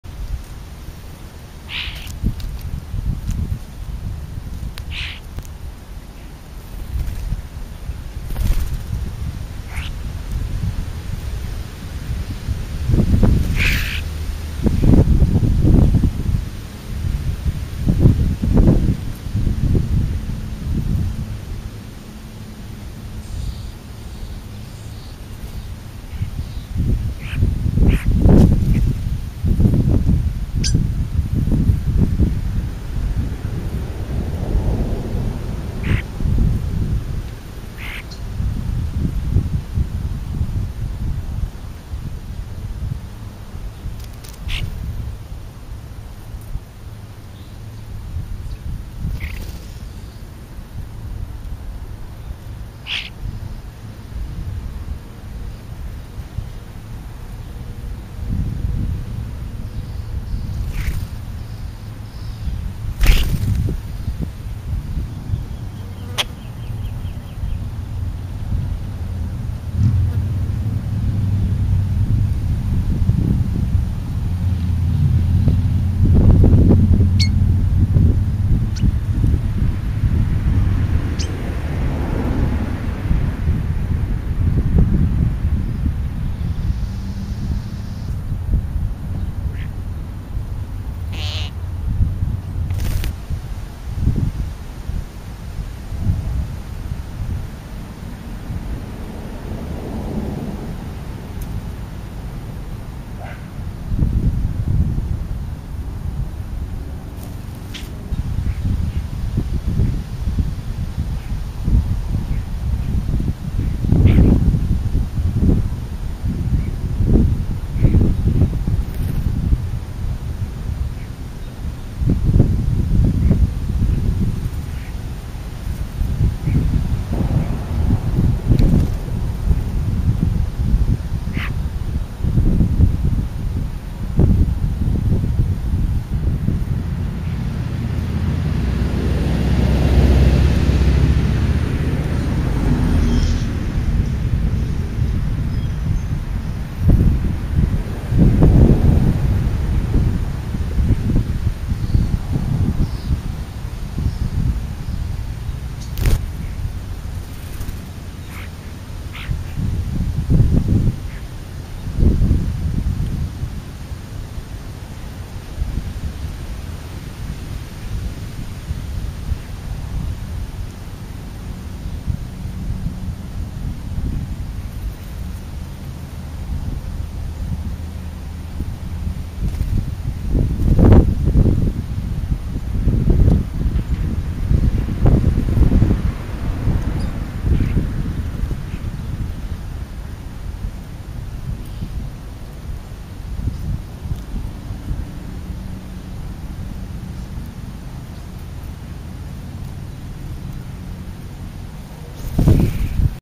Bird Feeding May 20, 2012 Part 3
French fries, ham lunch meat, chinese spegetti, scrambled eggs with cheeze, a brownie, and reeses pieces were all on the menu today. The crows really got into the eggs, but didn't eat anything else. After my camera ran out of batteries, I saw a crow eat a reeses piece.